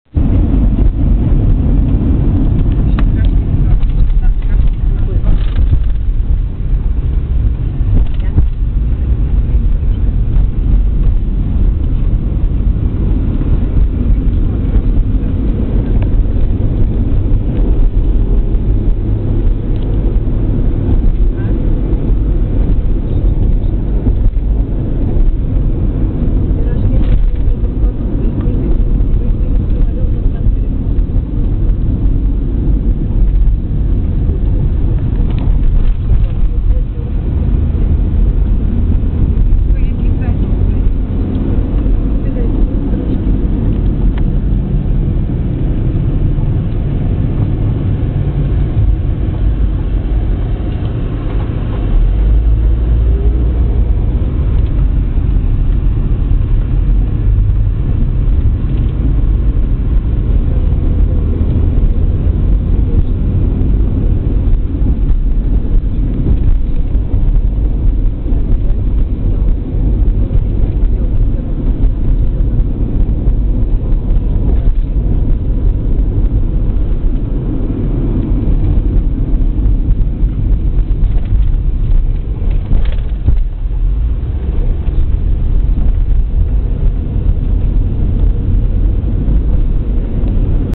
russian roads
Before you moan about potholes you should take a look at what our Russian comrades have to deal with. It's a real effort not to spill your vodka.